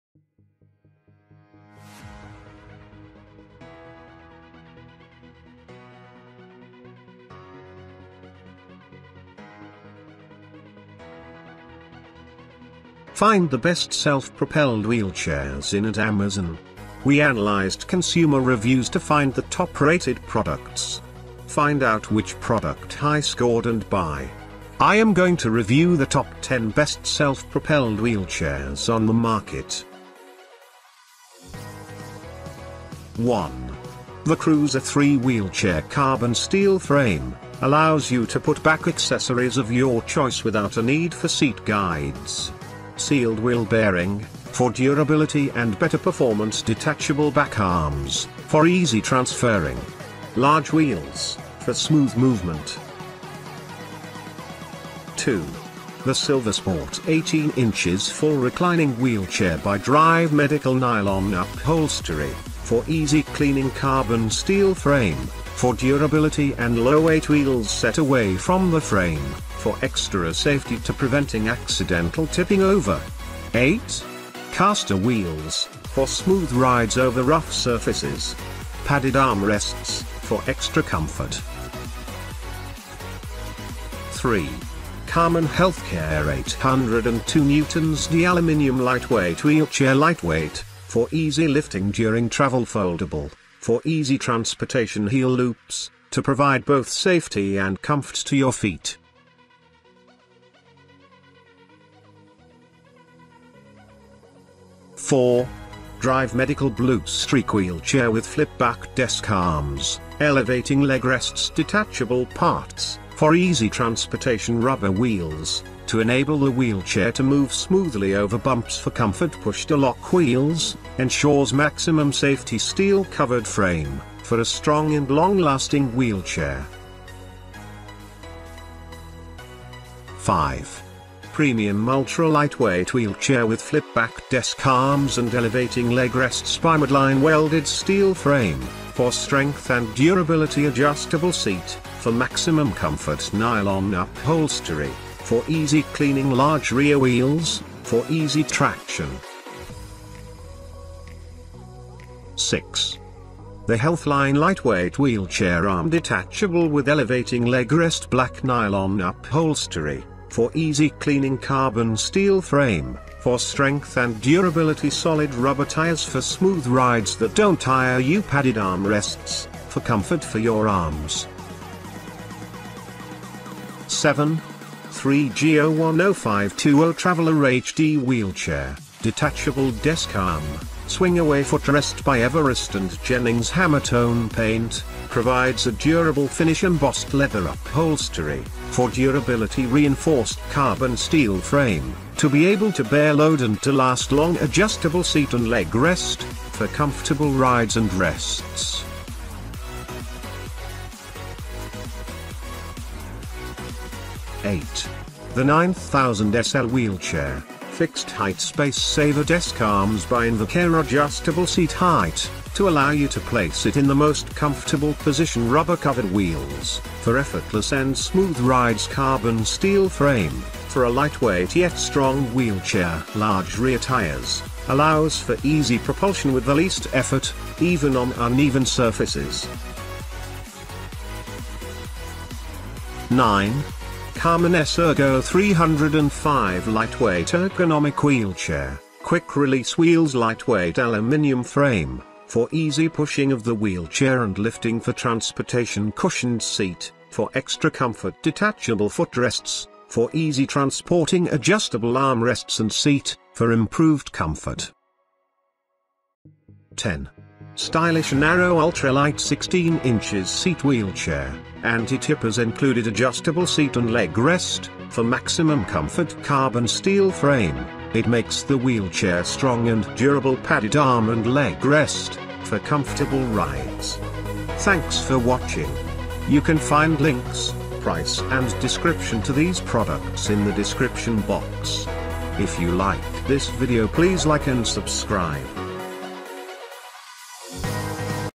Top 10 Best Self Propelled Wheelchair 2020
Hi guys, I am going to show video review on the Top 10 Best Self Propelled Wheelchairs on the market. Get your Best Self Propelled Wheelchair from here
Top 10 Best Self Propelled Wheelchairs